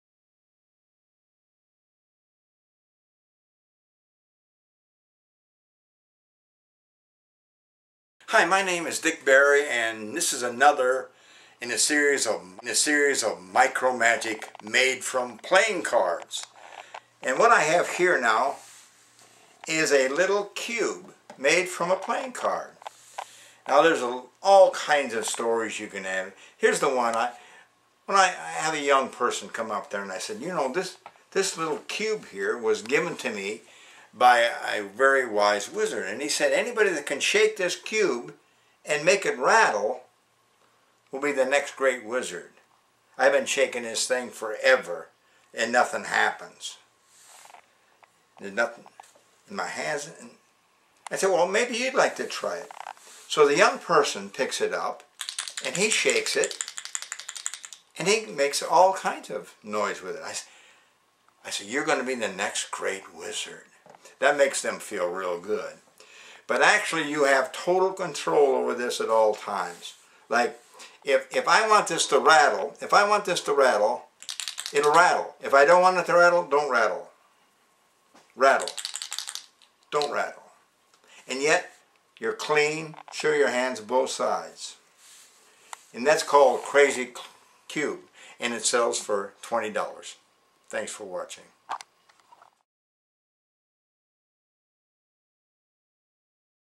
"CRAZY CUBE"
A TINY CUBE MADE FROM A PLAYING CARD RATTLES AND STOPS RATTLING AT YOUR COMMAND!